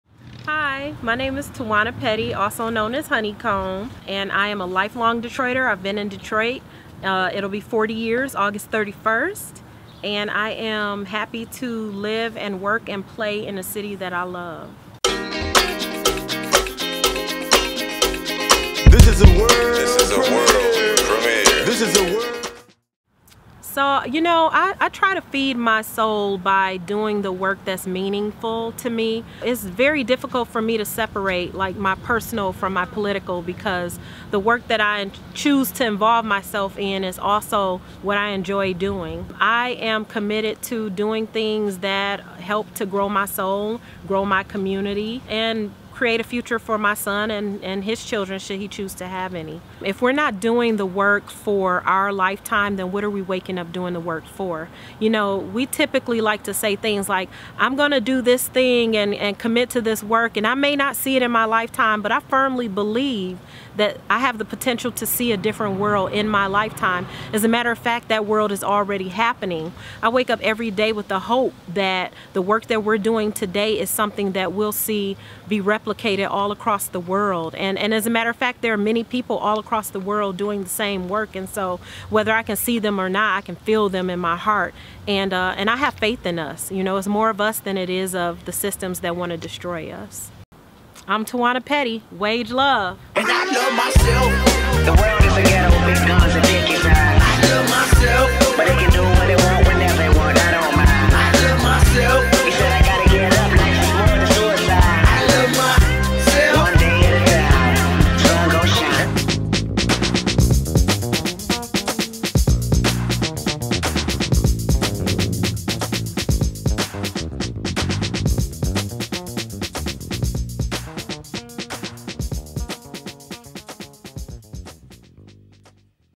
Feeding the Soul (w/ Tawana Petty)
Tawana Petty is a mother, anti-racist social justice organizer, youth advocate, poet and author. She was born and raised in Detroit, Michigan and is intricately involved in water rights, digital justice and visionary organizing work.  She is the past recipient of the Spirit of Detroit Award, the Woman of Substance Award, the Women Creating Caring Communities Award, the Detroit Awesome Award, and was recognized as one of Who’s Who in Black Detroit in 2013 and 2015.  Tawana is a board member of the James and Grace Lee Boggs Center to Nurture Community Leadership, a Data Justice Community Researcher for the Detroit Community Technology Project, a Detroit Equity Action Lab (DEAL) Fellow, a member of Detroiters Resisting Emergency Management, and the The Detroit Digital Justice Coalition.